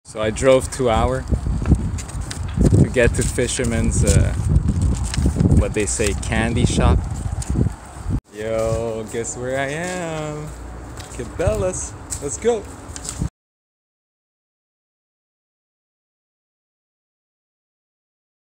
Went to Fisherman's Candy Shop (Cabela's/Bass Pro Shops) for new Musky Baits!!
Just a short fun video of when I drove 2hrs to go to the closest Cabela's/Bass Pro Shops which is located in Ottawa, Ontario.

I went there to have a look what they have in store and end it up buying some new musky baits for the fall season!! Baits varying from 9 to 14 inches!!

Musky video coming really soon!!